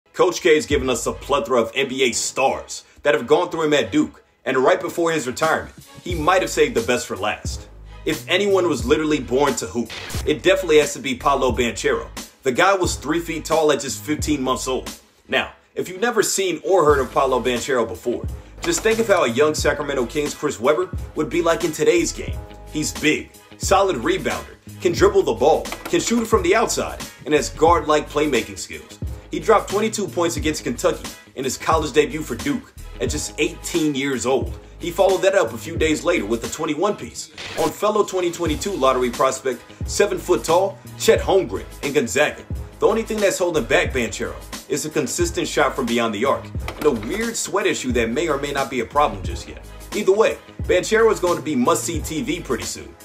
Coach K’s FINAL Gift ❤ | Clutch
Throughout his coaching career, Coach Mike Krzyzewski has blessed the basketball world with several future NBA stars, like Kyrie Irving, Grant Hill, Zion Williamson, and JJ Redick. And with the 2021-22 NCAA season being his last, he left one final gift to the basketball world: Paulo Banchero.